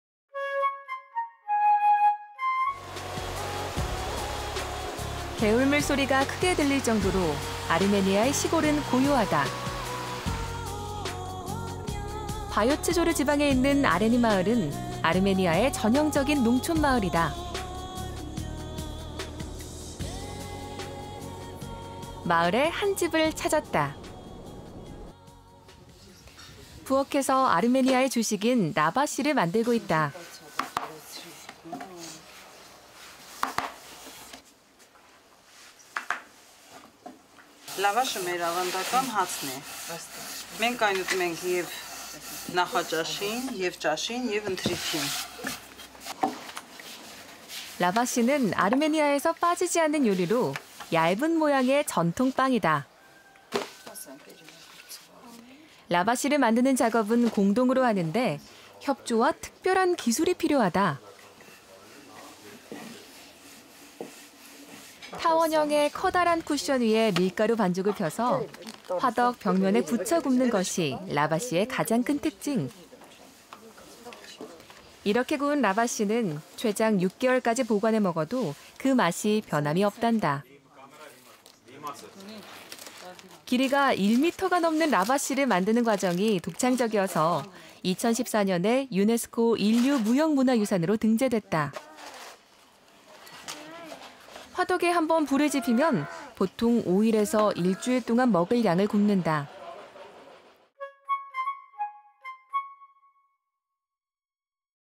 Armenia travel with KBS: Baking Lavash in Areni KBS와 함께하는 아르메니아 여행: 아레니에서 전통 빵 라바시 만들기
ARMENIA TRAVEL WITH KBS: BAKING LAVASH IN ARENI (IN KOREAN)

If anything identifies the Armenian cuisine, it is the traditional form of bread, called Lavash (very large oblong thin bread made entirely by hand and baked in stone or clay ovens buried in the ground). Lavash especially delicious warm from the oven wrapped with cheese and fresh greens.

Lavash, the preparation, meaning and appearance of traditional bread as an expression of culture in Armenia is inscribed in 2014 on the Representative List of the Intangible Cultural Heritage of Humanity (UNESCO)

You can always join Phoenix Tour to observe the thousand-year-old tradition of Lavash making as it is done in the villages of Armenia.

Video credits: KBS